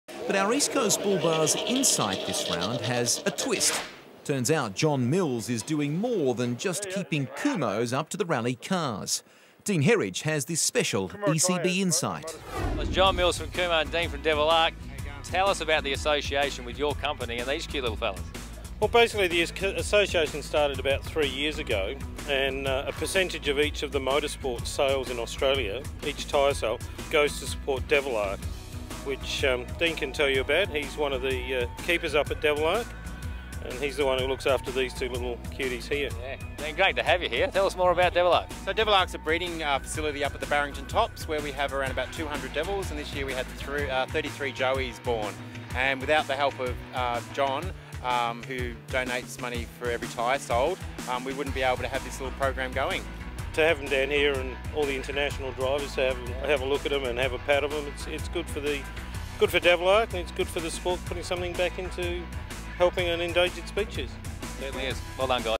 ECB Insight - Tassie Devils
On a lighter note of rallying, John Mills from Kumho talks to Dean Herridge about his long standing partnership with Devil Ark.